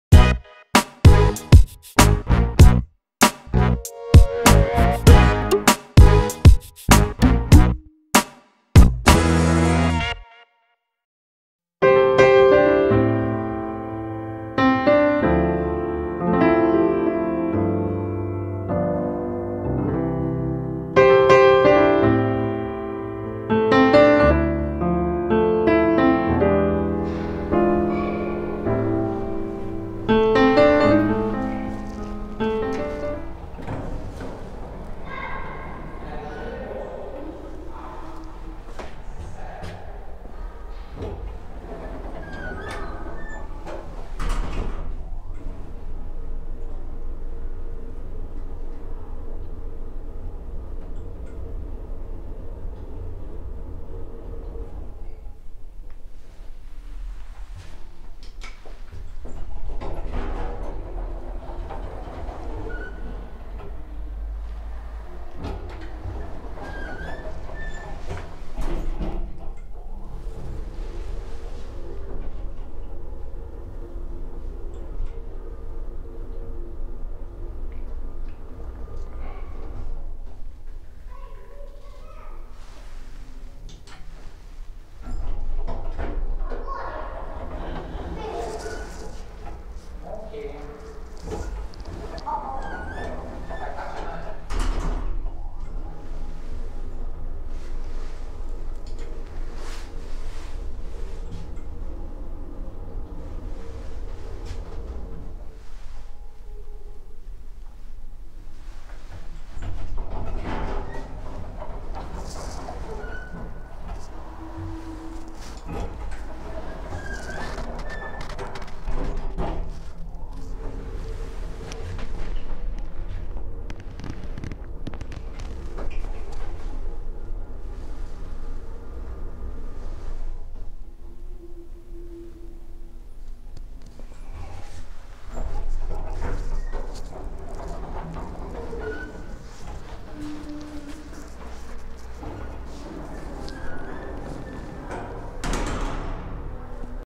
OTIS Europa 2000 Aufzug in einem Ärztehaus in Krefeld-Uerdingen
Hier filme ich einen OTIS Europa 2000 Aufzug in Krefeld-Uerdingen.

Here i filmed an OTIS Europa 2000 elevator at Krefeld-Uerdingen.